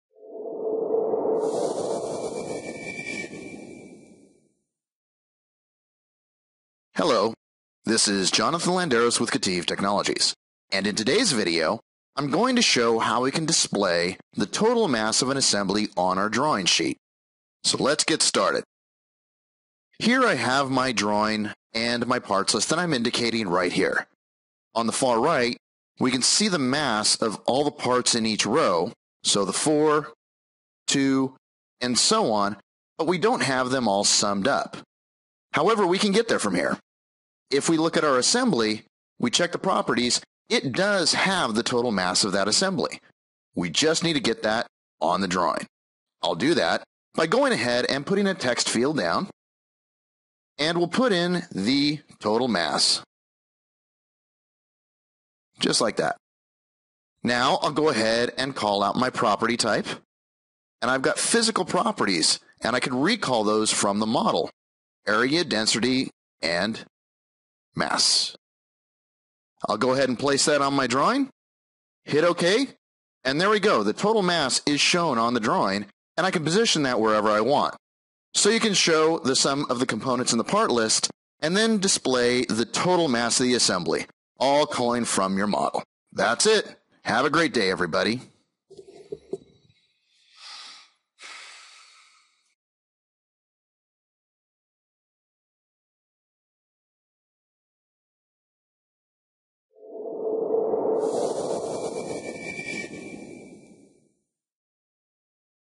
Showing Total Mass on an Autodesk Inventor Drawing
This video suppliments the previous video "Adding Up Parts List Values" from February 12, 2012.

In this new video, Jonathan Landeros of KETIV Technologies shows how to add the property that displays the total mass of an assembly on the drawing.